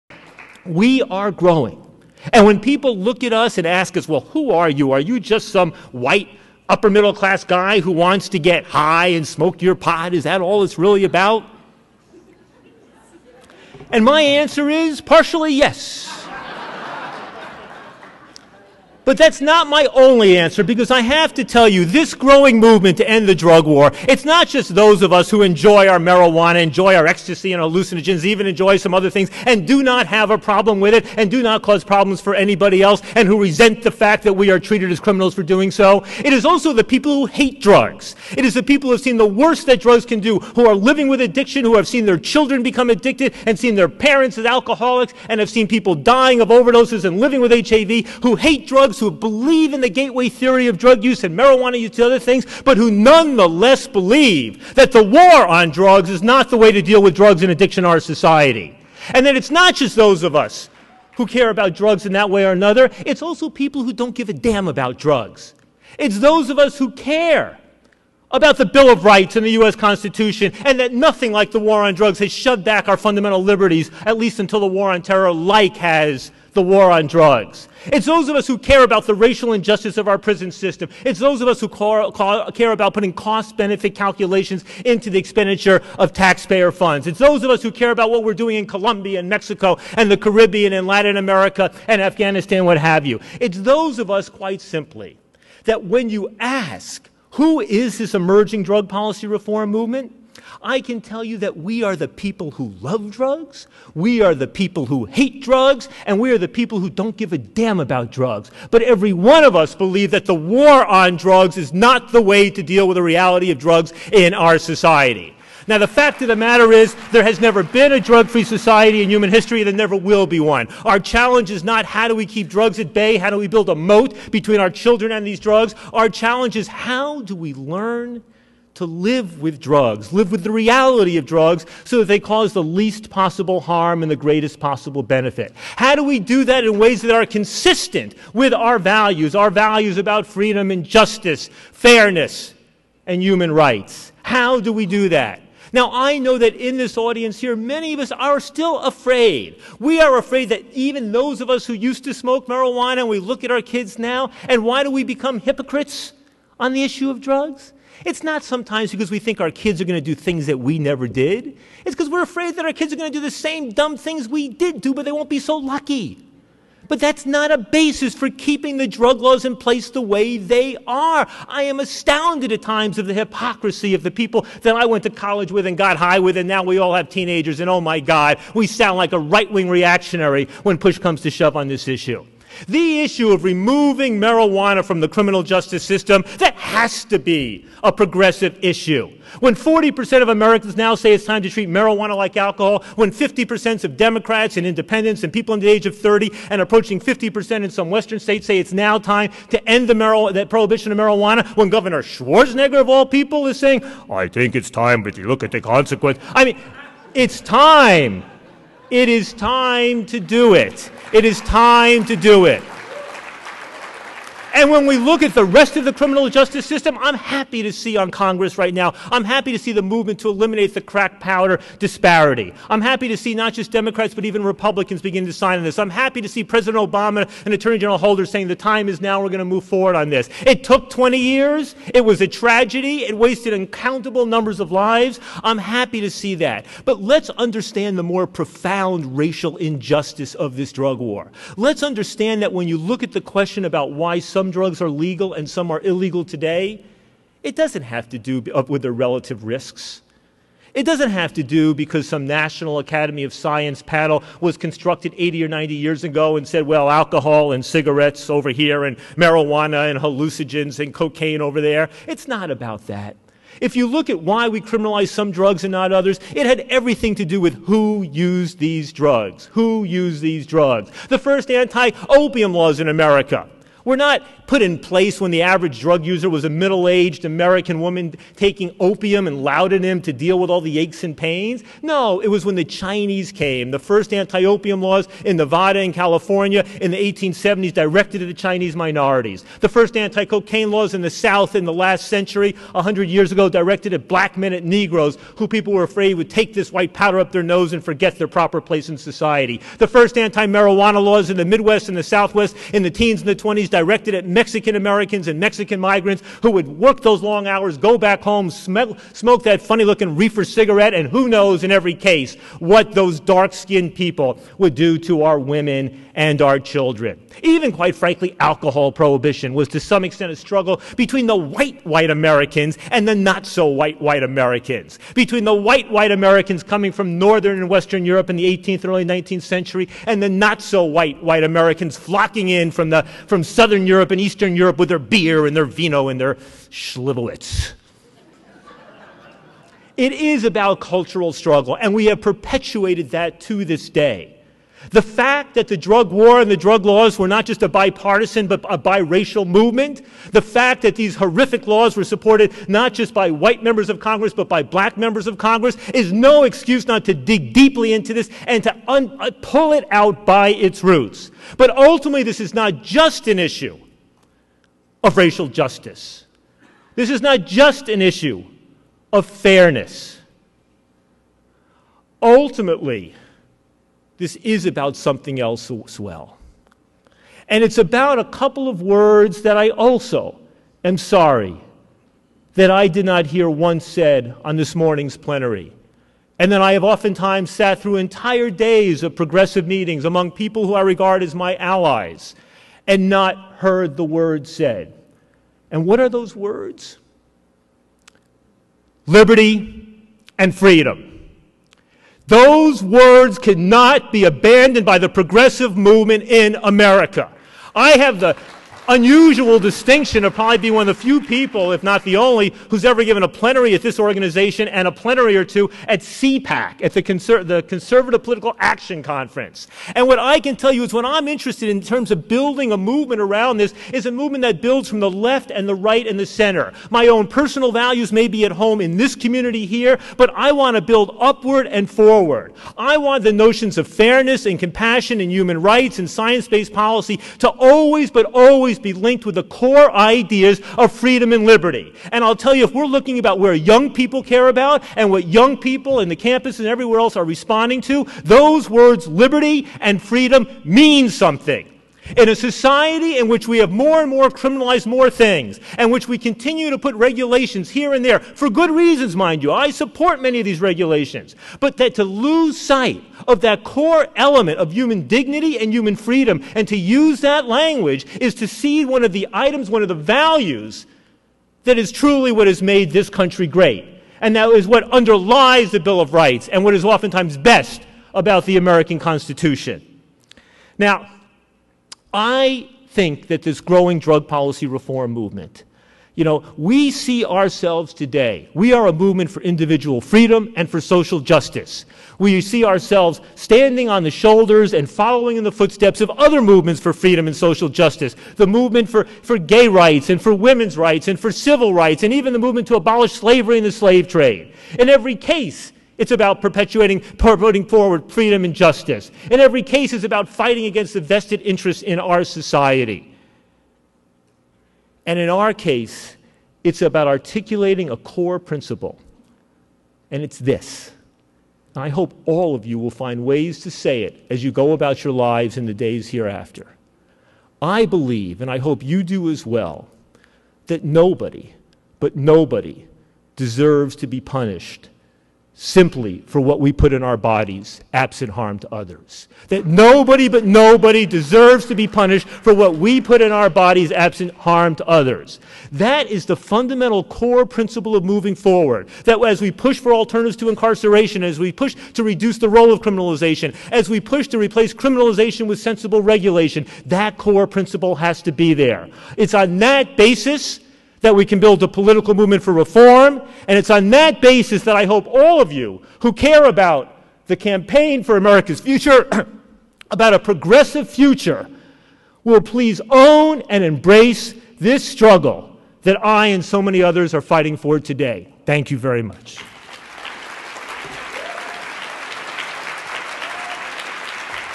Ethan Nadelmann: "End the Failed War on Drugs" (2 of 2) Tides Momentum @ AFN 2009
Session: Momentum@America's Future: POWER Session

Participants:
Lawrence Lessig
Melissa Bradley-Burns
Ethan Nadelmann
Drummond Pike